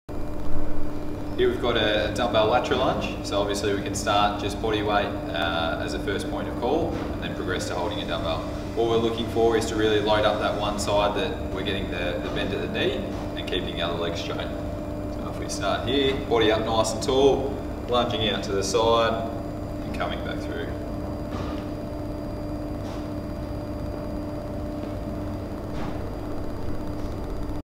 Dumbbell Lateral Lunge
How to perform a Dumbbell Lateral Lunge with Strength and Conditioning Coach Tim Welsh.